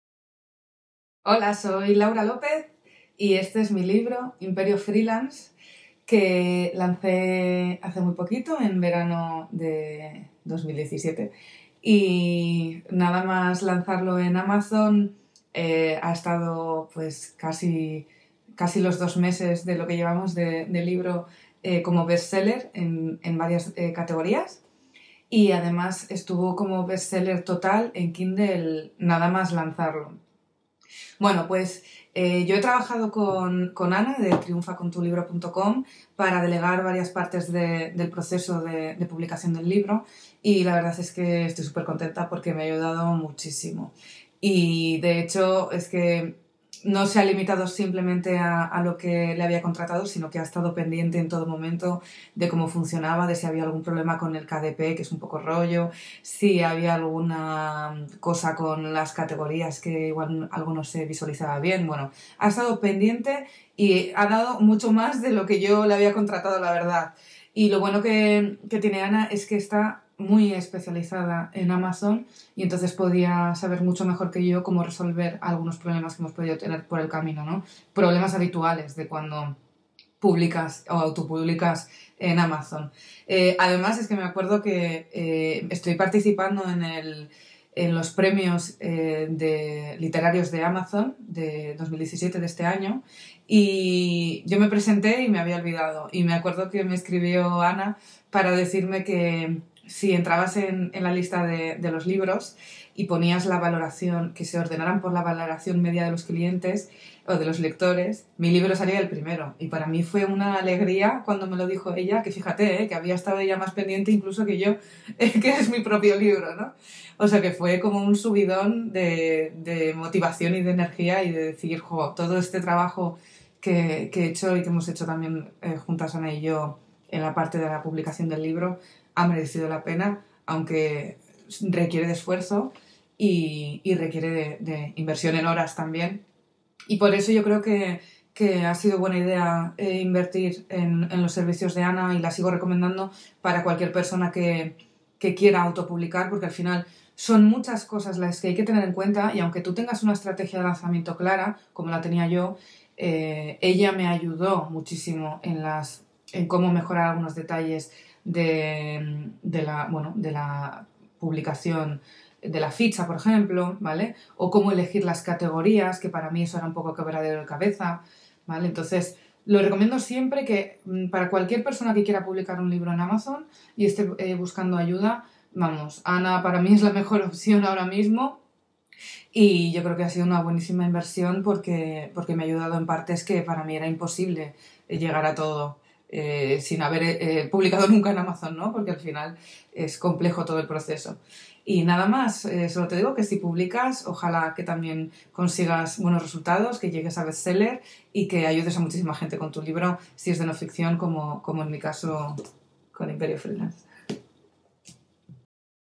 Autopublicar en Amazon y ser bestseller: Testimonio de Laura López
Laura López ha sido autora bestseller entre los más de 300.000 ebooks publicados en español en Amazon. Nos cuenta sus éxitos, sus sensaciones y cómo le ayudamos a conseguirlo.